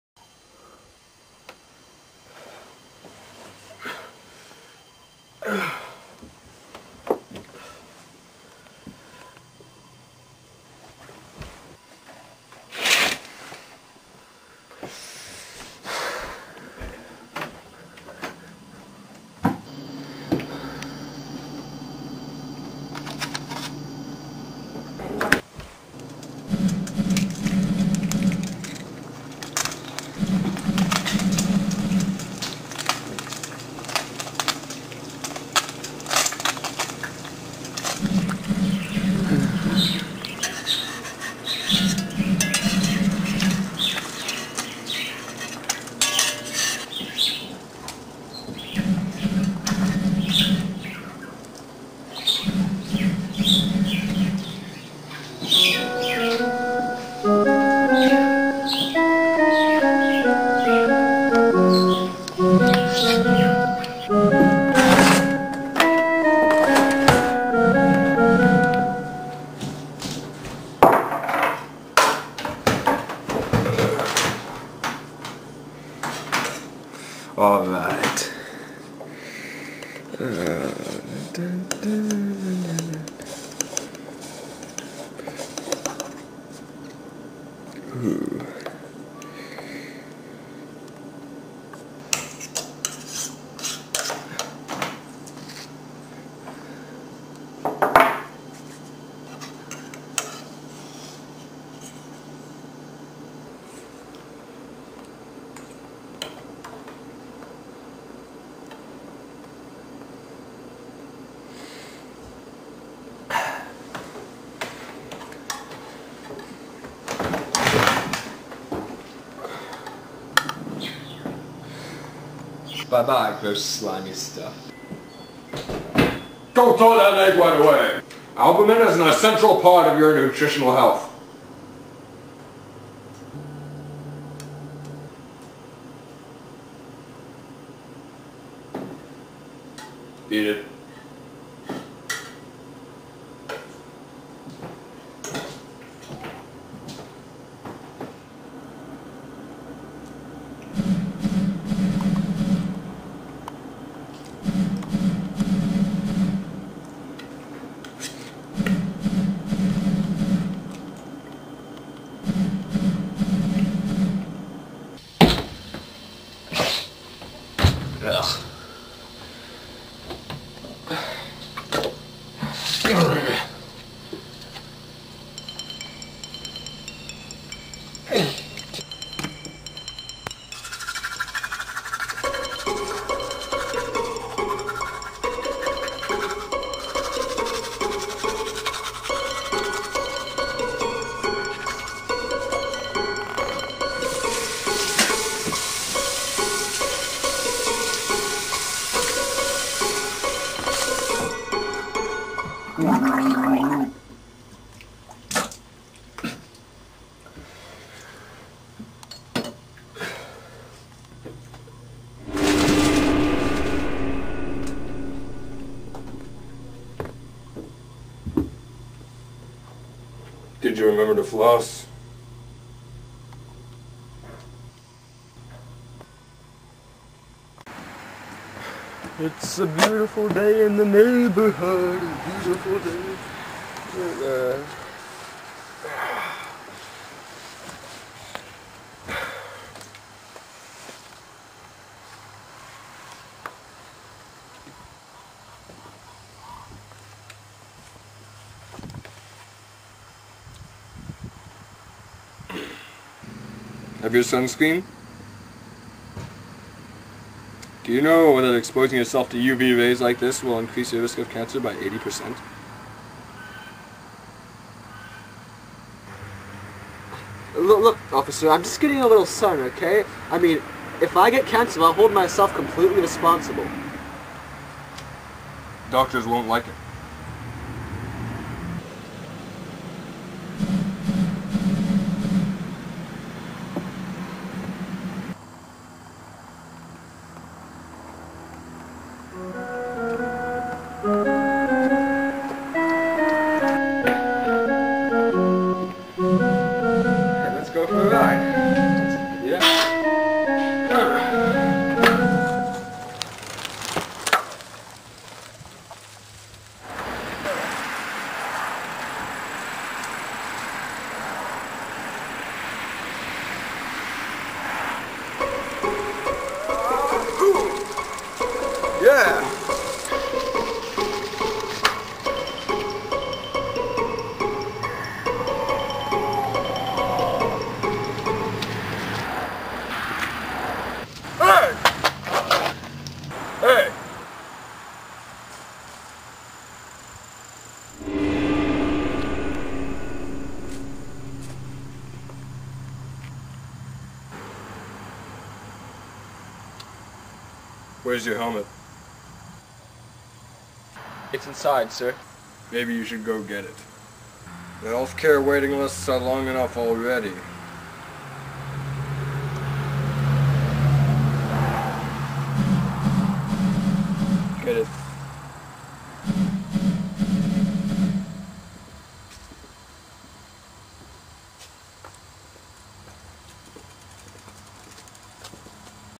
Hope-topia Part 1
Take a good look: this is daily life in Post-Obama America. Starring Chris and Joe Herring, Directed and Edited by Joe Herring.